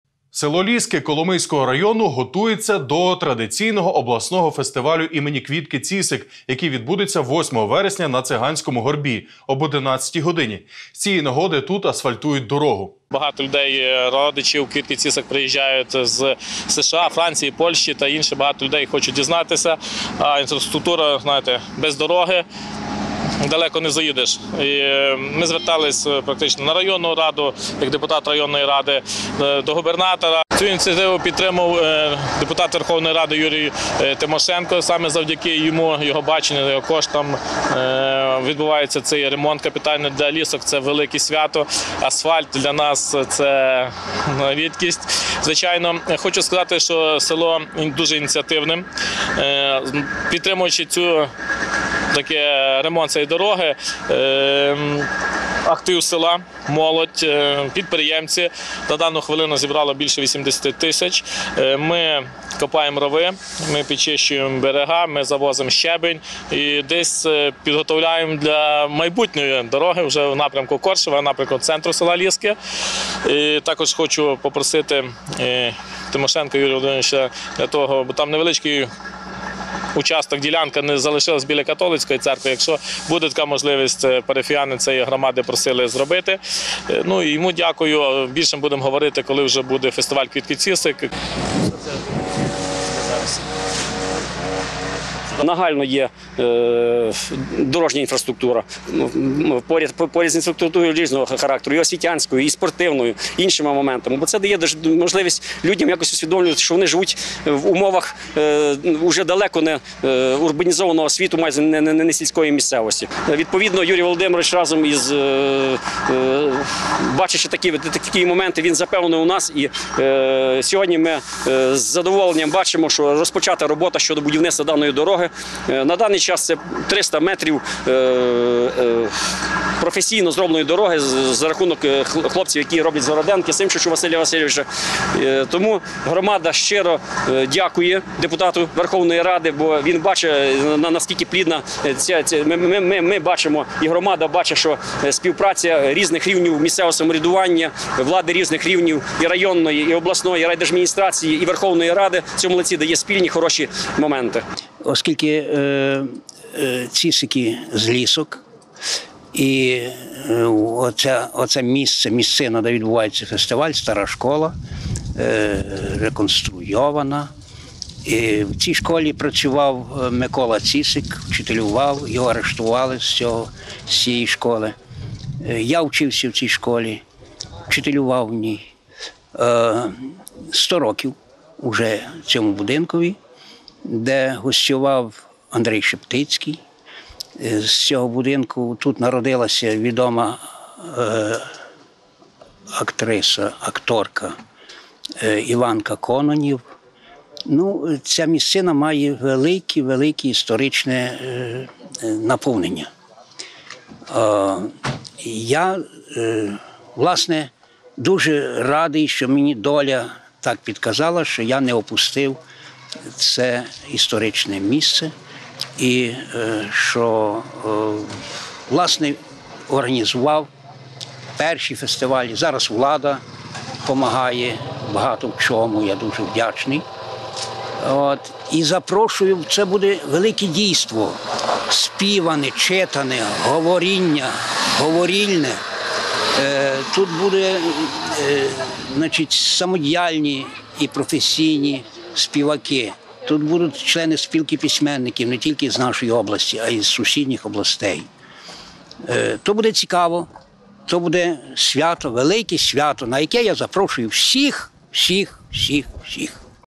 На Коломийщині готуються до обласного фестивалю імені Квітки Цісик
Село Ліски, Коломийського району готується до традиційного обласного фестивалю імені Квітки Цісик.

Захід відбудеться 8 вересня на Циганському горбі.

Початок об 11:00 годині.

З цієї нагоди тут асфальтують дорогу.